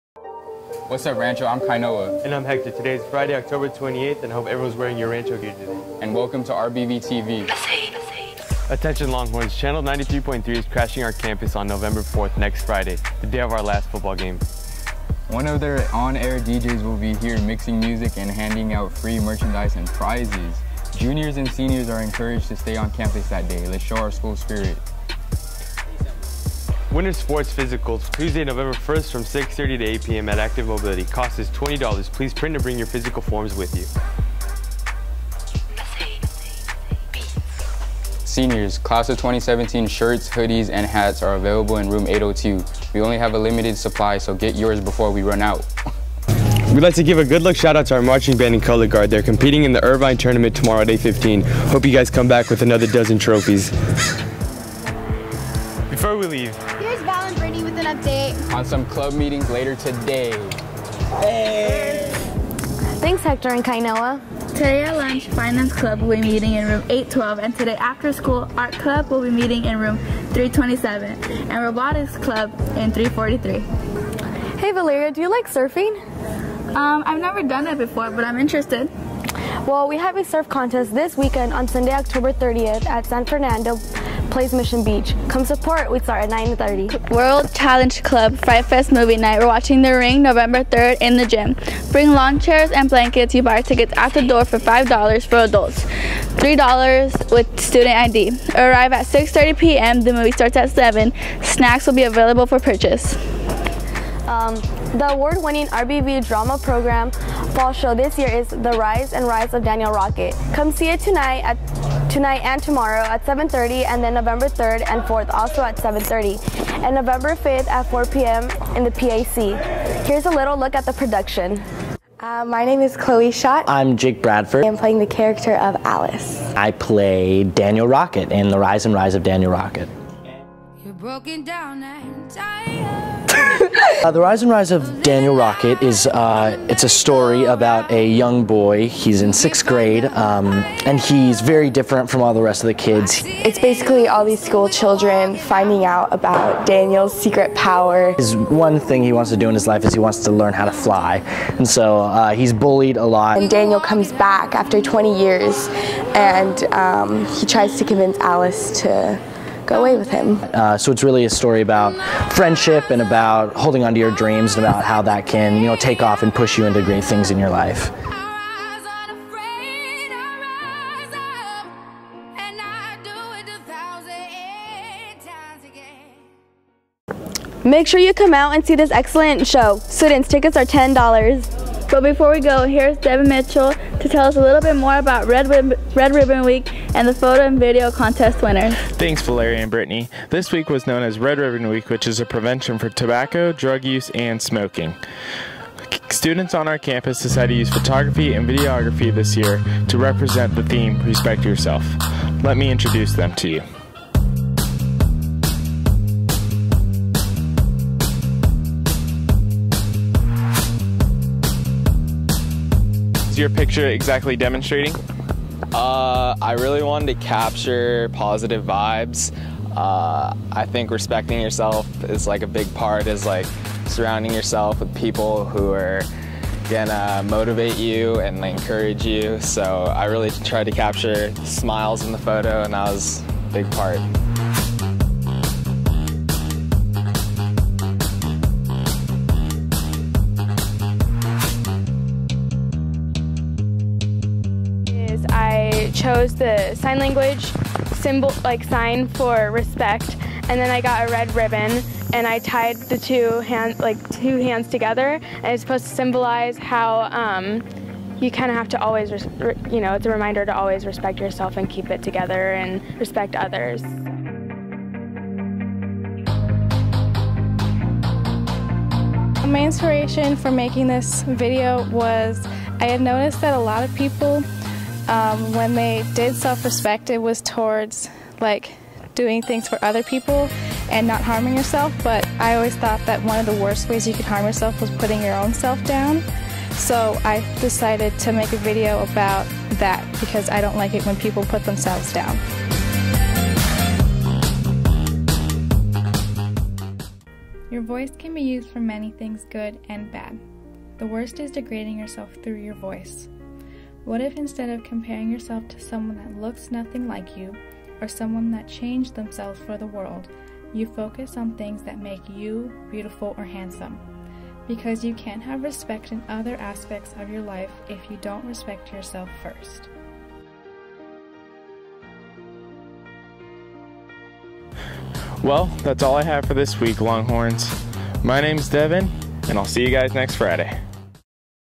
RBVTV 10-28-16 with Fall Drama Show Preview
& Red Ribbon Week Contest Winners